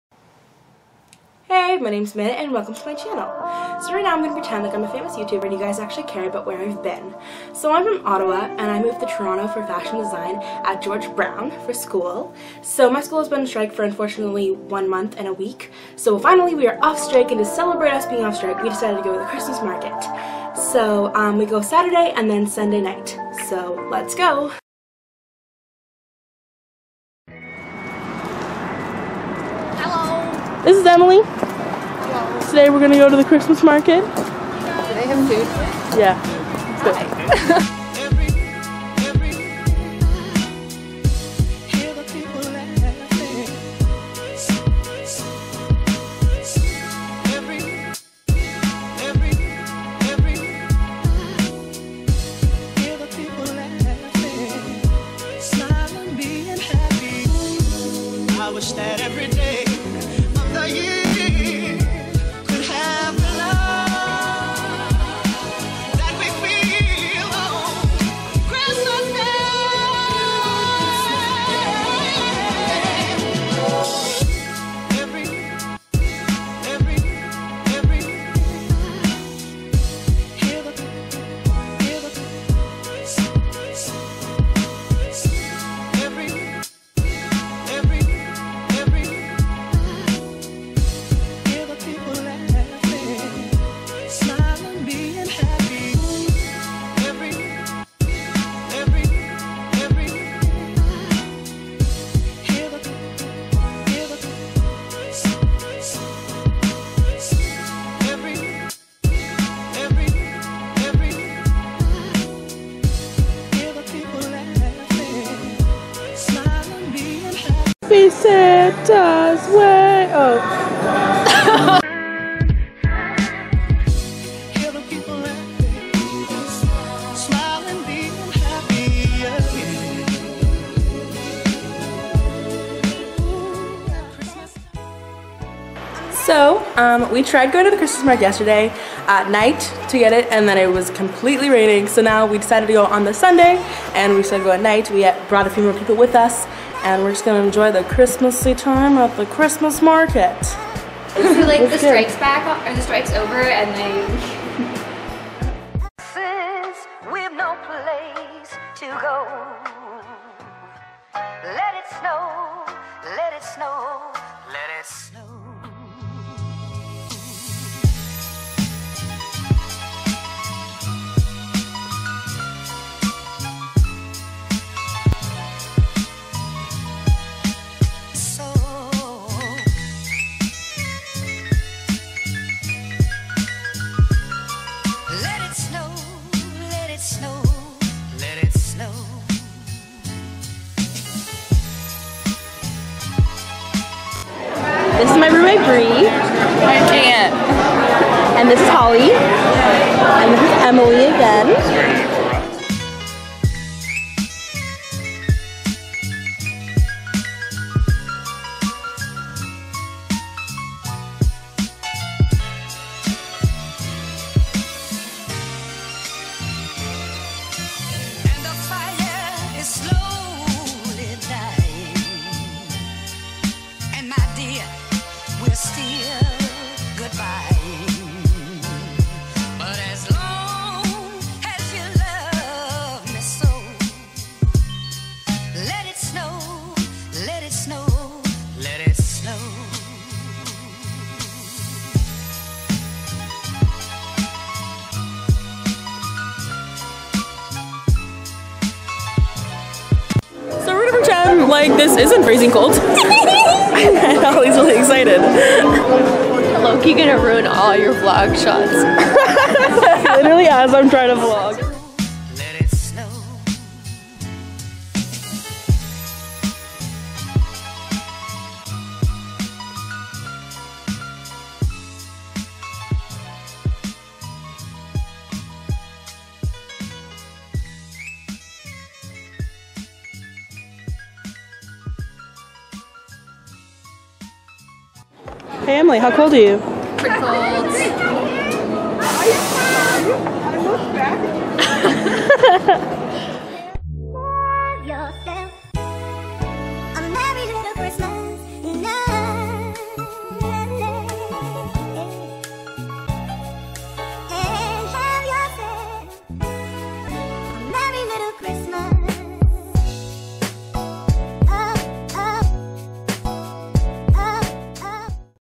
Toronto Christmas Market
Come join my friends and I enjoy the Christmas market in the Distillery District! The Christmas market will be open until Christmas and it is 6$ to enjoy all the festivities they have planned!